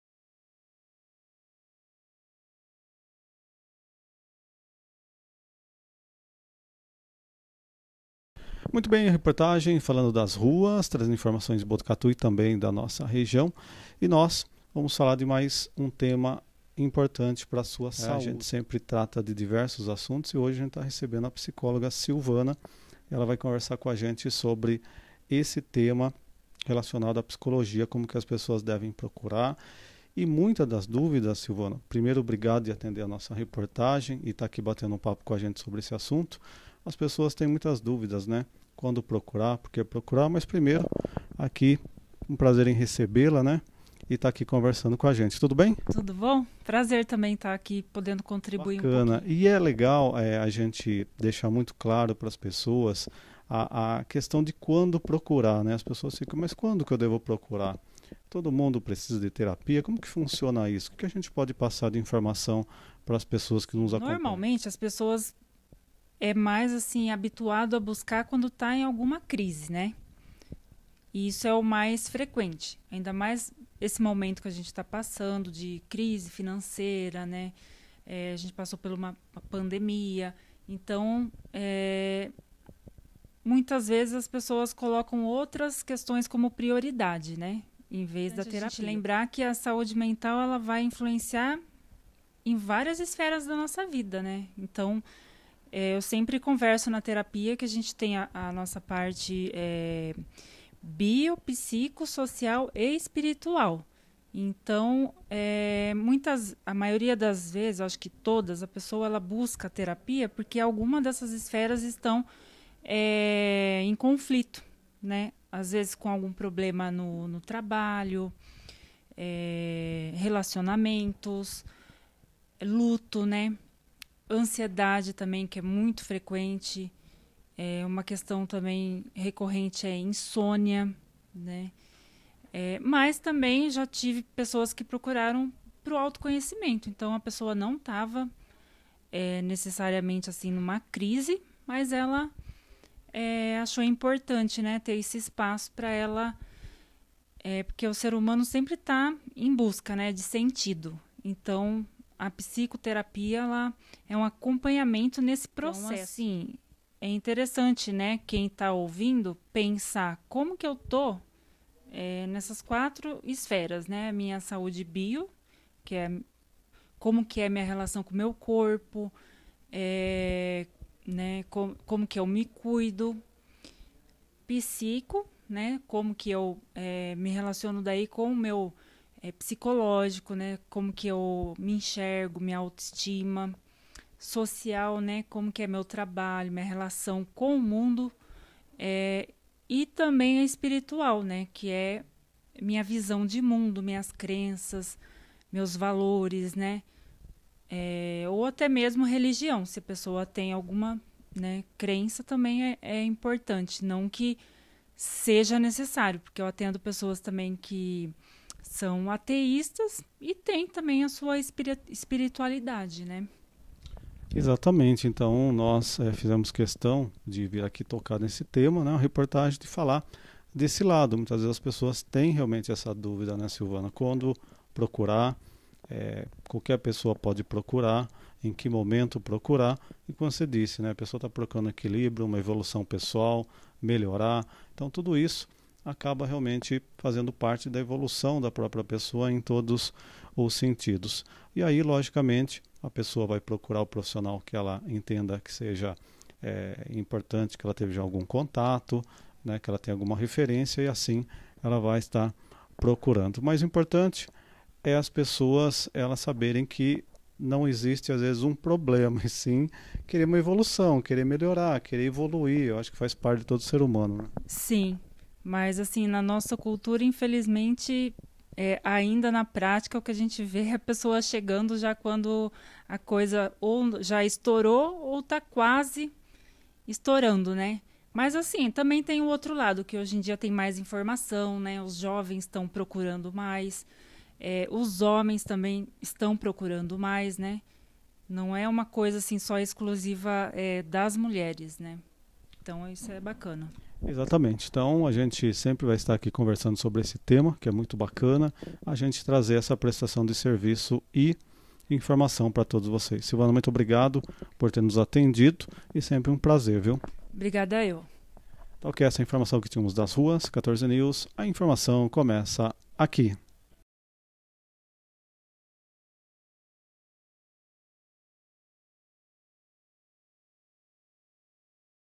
Psicologia: quando procurar terapia
Conversamos com a psicóloga Silvana Hilario (Especialista em Saúde Mental) que atua em Botucatu e com atendimento online.

Você tem dúvidas?